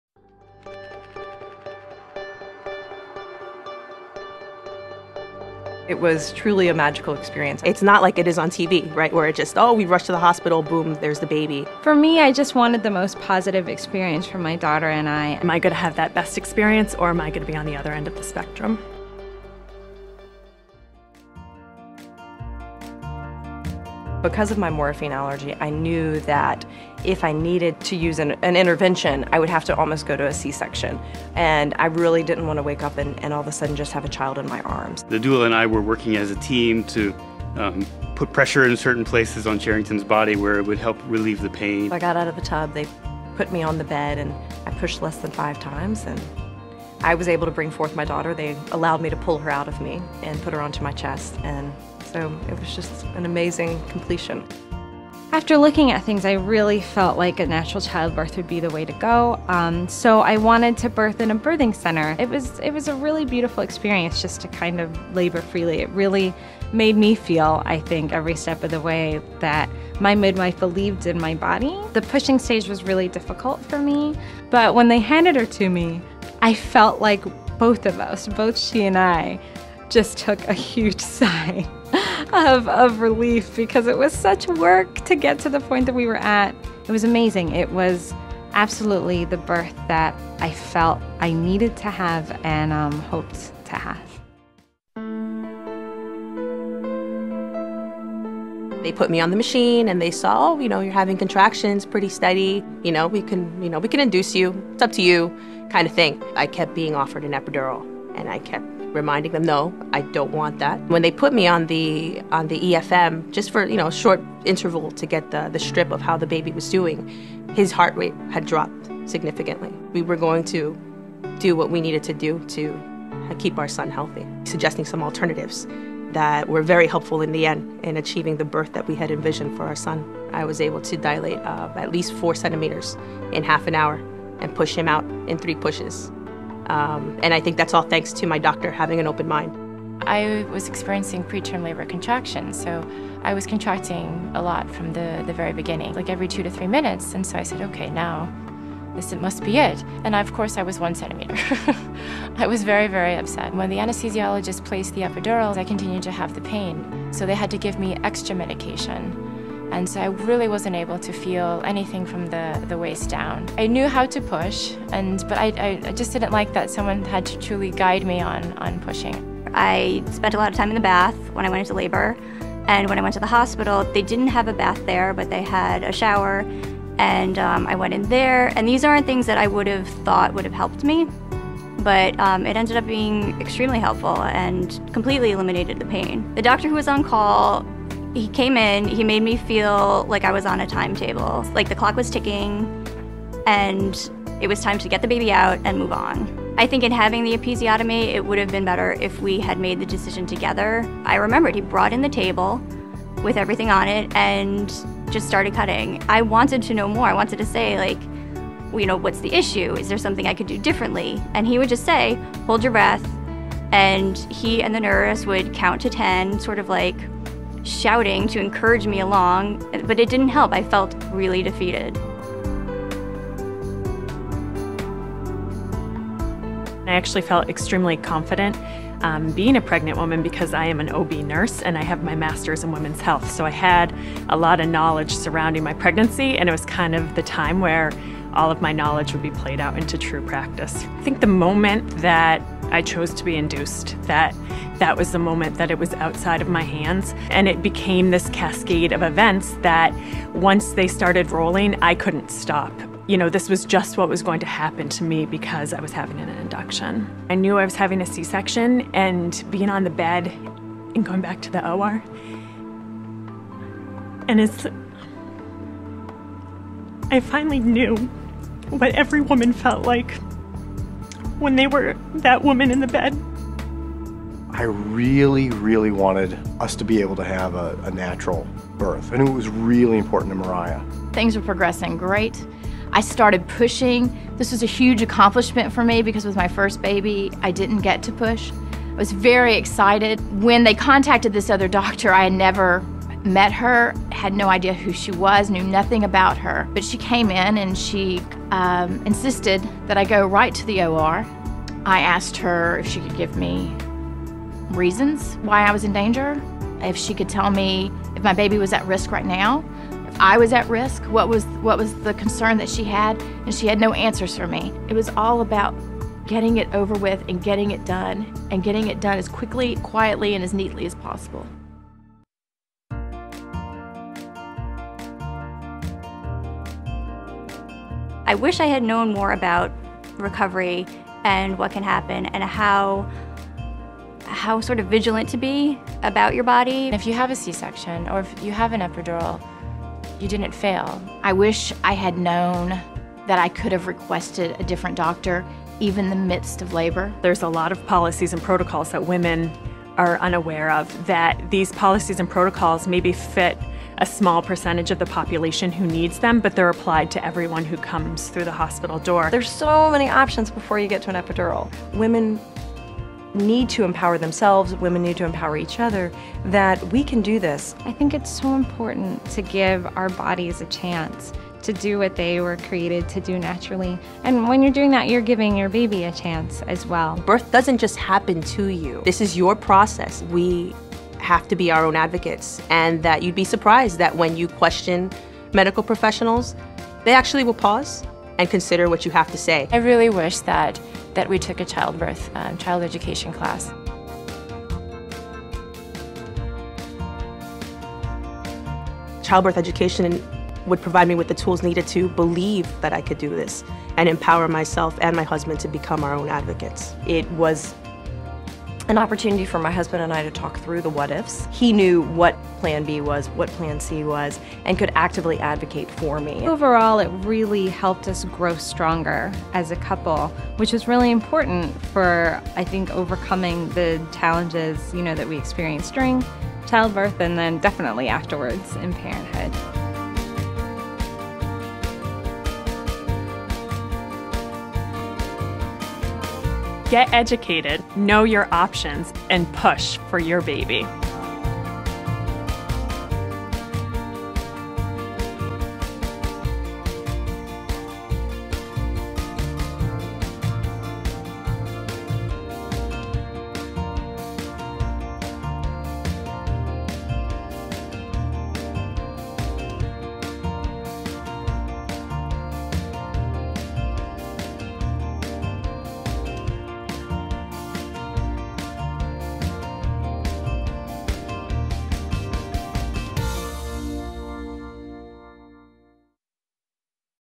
Push for Your Baby - Parents Share Their Stories
Seven women share their personal childbirth experiences, including the highs and lows, and underscore the importance of childbirth education to having the safest, healthiest birth possible. Get the tools and information you need to push for your baby